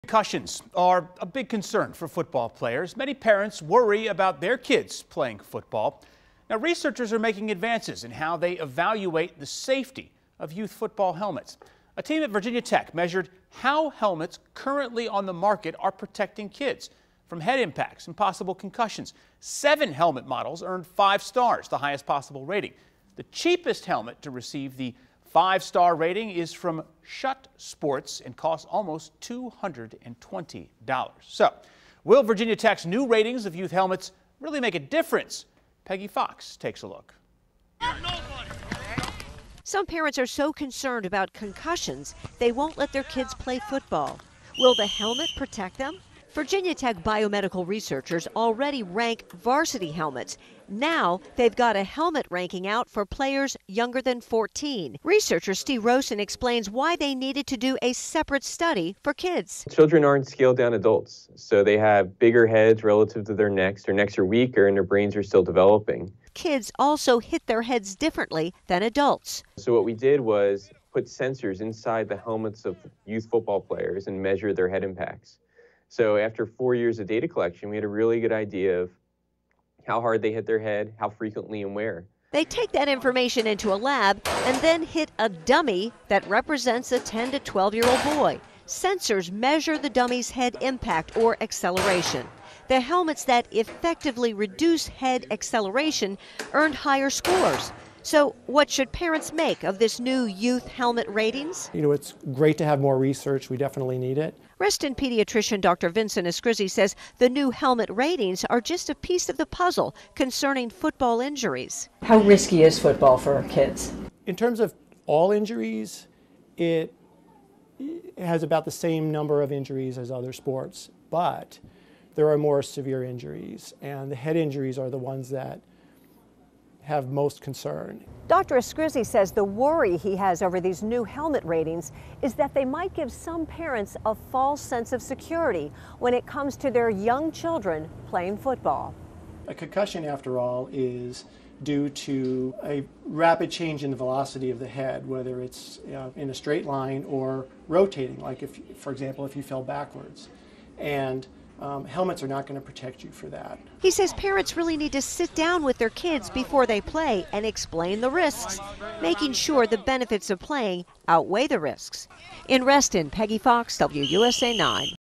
Will youth helmet ratings make football safer?
Some parents are so concerned about concussions, they won't let their kids play football. Will the helmet protect them?